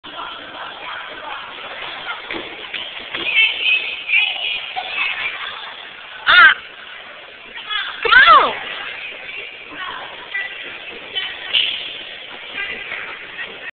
NeW ORLeaNS BounCE : HeaDSTaND
it wusz me at da dam mall of the mainland bein stupid...plus i wusz messin wit a cop cusz he tried to say we wusz stealin out of CLAIRE'S??!!!. come on nah claire's??
but dats aight cusz i got sumtin 4 yuh ***